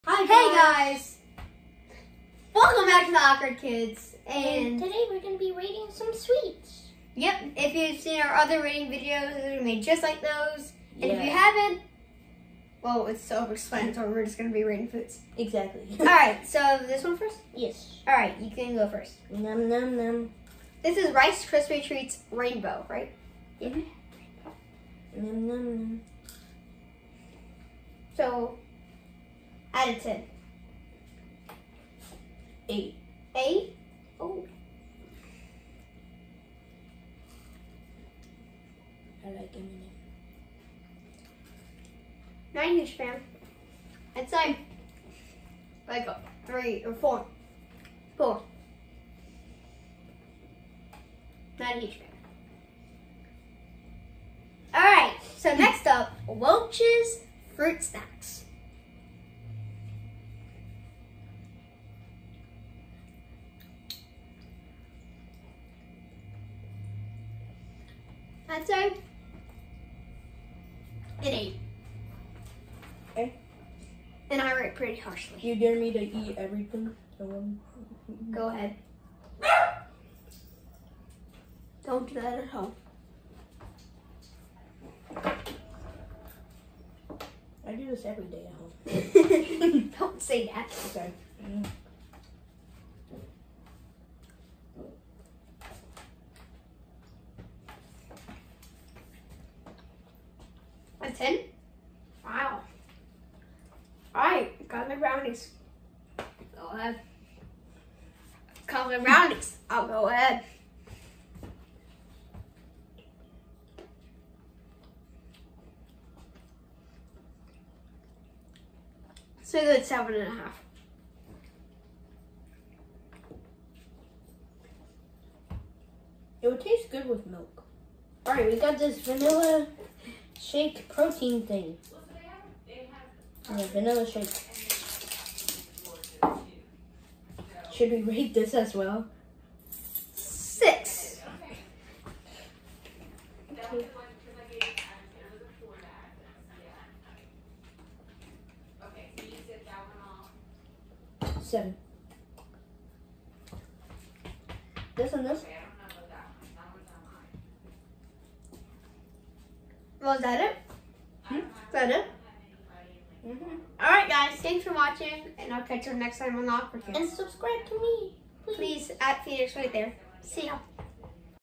Rating Sweet Foods
Today me and @9hoenix will be rating sweet foods.